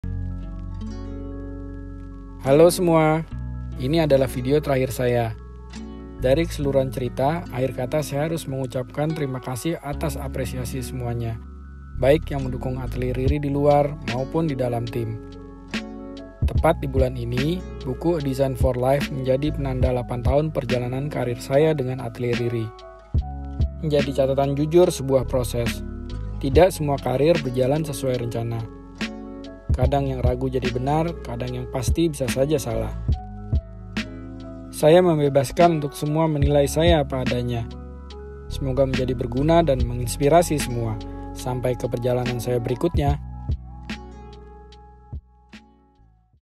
A Design for Life (eps.09 FINALE): Happy 8th Anniversary
A Design for Life book 2018

Tepat 1 tahun yang lalu saya dan Atelier Riri mengeluarkan buku kedua yang berisi tentang cerita perjalanan karir dan kumpulan proyek arsitektur terpilih sejak sebelum mendirikan Atelier Riri sampai tahun 2018.

Buku ini menjadi penanda bagian dari perjalanan arsitektur saya dengan Atelier Riri. Semoga cerita di dalamnya dapat menginspirasi dan memberikan wawasan baru untuk orang yang membacanya.

Yang ingin mendapatkan buku ini (mungkin) masih tersedia di gerai2 toko buku seperti Gramedia, Gunung Agung, Paper Clip, dsb.

Video ini sengaja saya dokumentasikan untuk mengabadikan cerita  dan visi di balik pemikiran dan latar belakang saya dalam berkarya.

Terima kasih kepada:
- Ren Katili (book launching coordinator)
- Yoshua T.H (videgrapher & editor)